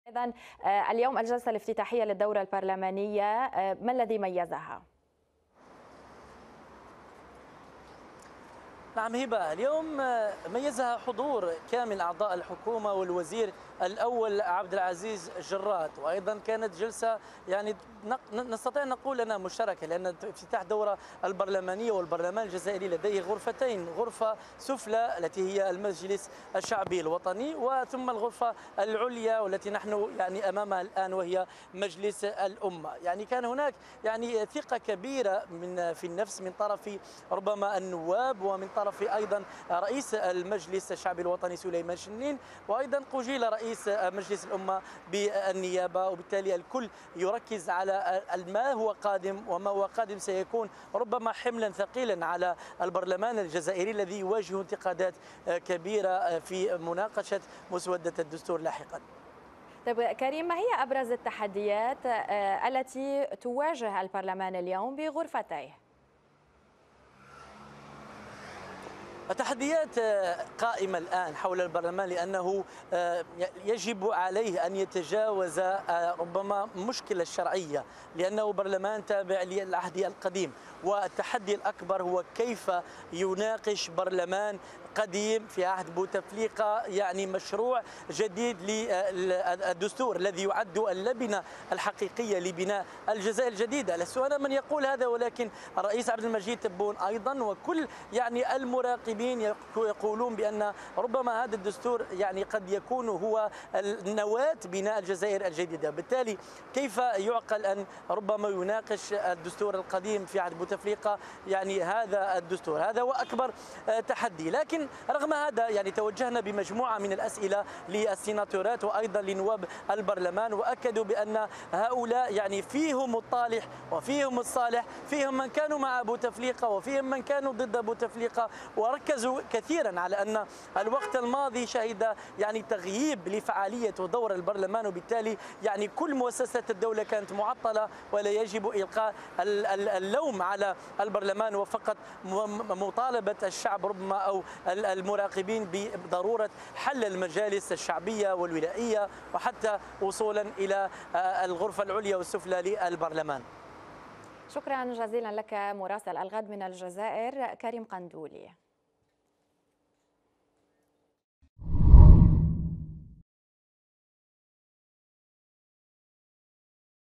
هذه أبرز التحديات التي تنتظر البرلمان الجزائري
افتتح البرلمان الجزائري دورته العادية الجديدة اليوم.

وأفاد مراسل الغد بأن البرلمان يعقد جلسة مشتركة بغرفتيه وبحضور الحكومة لمناقشة مسودة الدستور.

المزيد من التفاصيل يتحدث مراسل قناة الغد من الجزائر، كريم قندولي، عبر النشرة الإخبارية